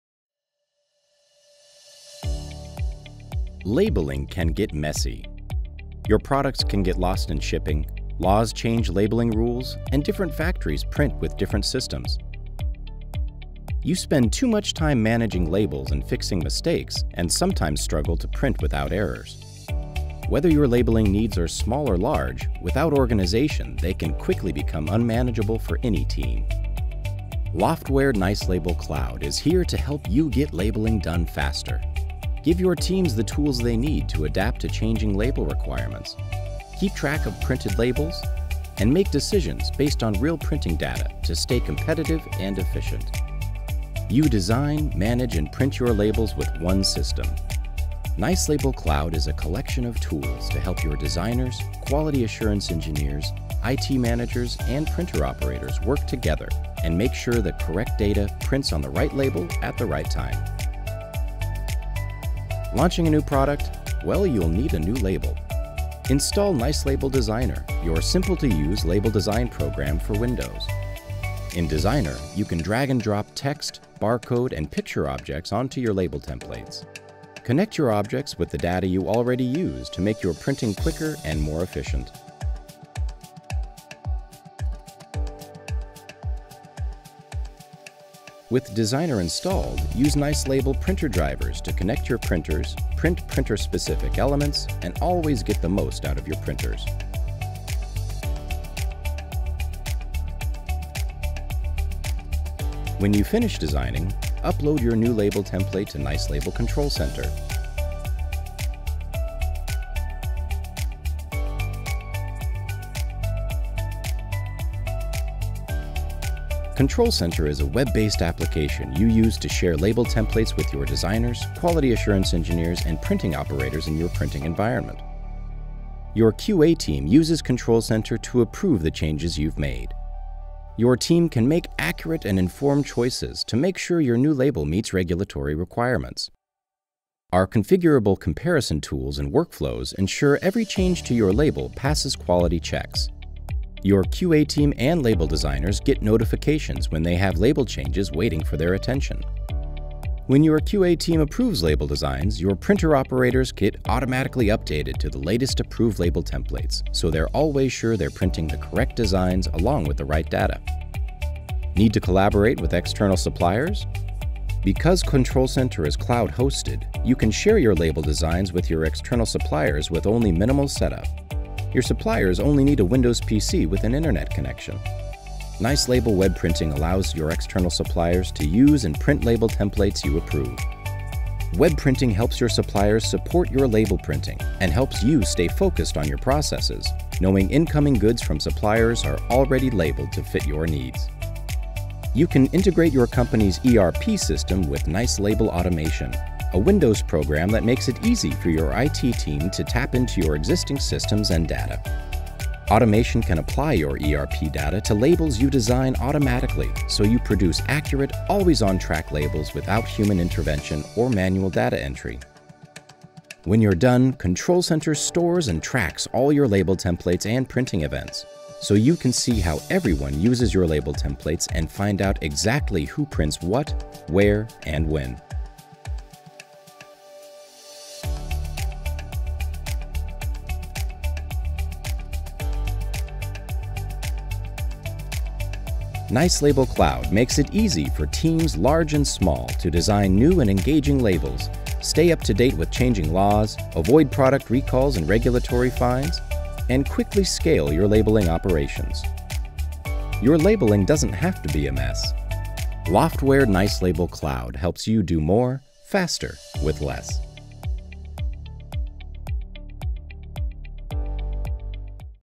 Loftware Cloud Introduction
Loftware Cloud is a cloud-based version of our hosted Label Management System that allows you to design, print, and manage labels. Some components are fully web-based, others work with software you download to your Windows PC or Mac.